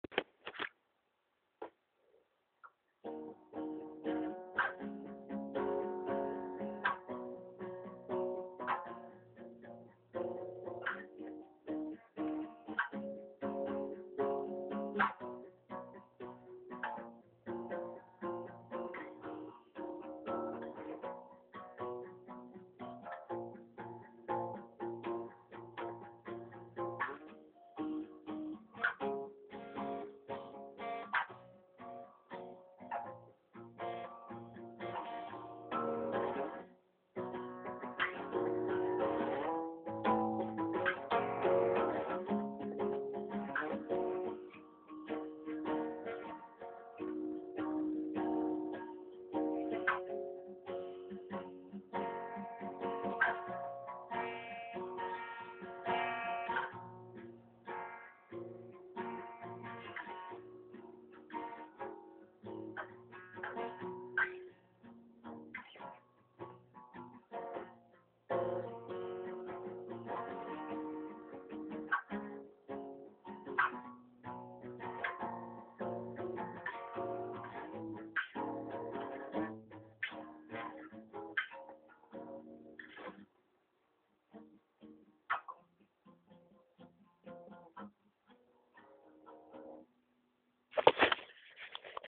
learning guitar
attempting to play guitar..quite shyly.. i'll get better (i hope!)